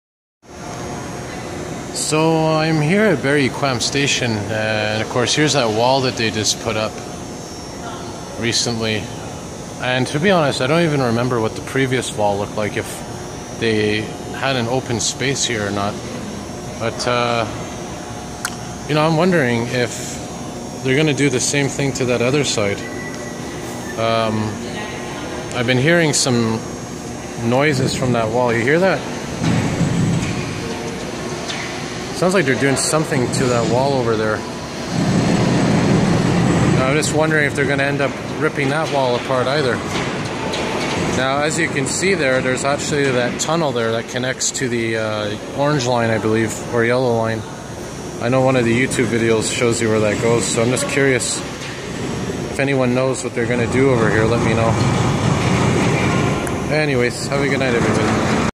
Berri-UQAM wall update
Looks like the wall they were working on is almost finished.